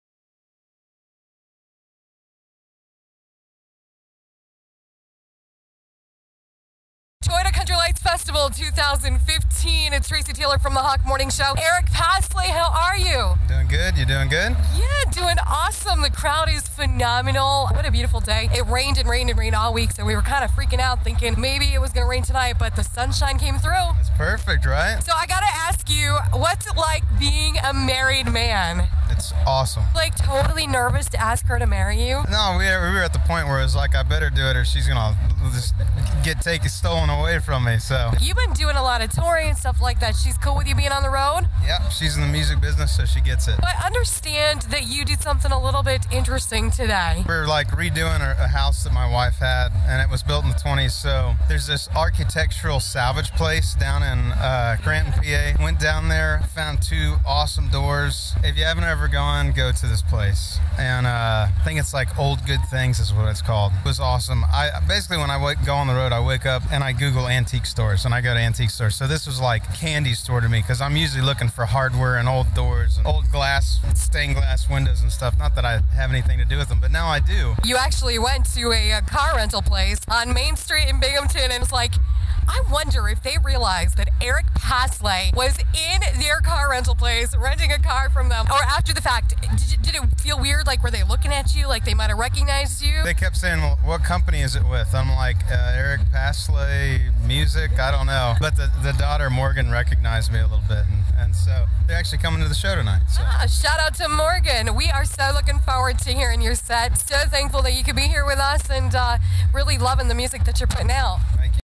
Eric Paslay Explains Why He Hunts for Antiques
Eric Paslay chatted with us about why he loves to hunt for antiques.